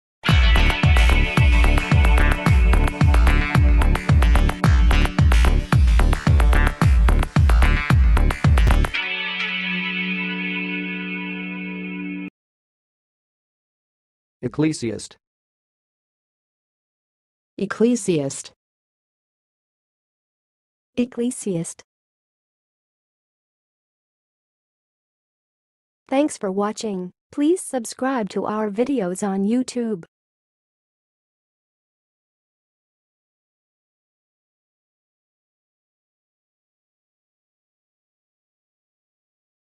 How to say "ecclesiast"! (High Quality Voices)
Watch in this video how to say and pronounce "ecclesiast"!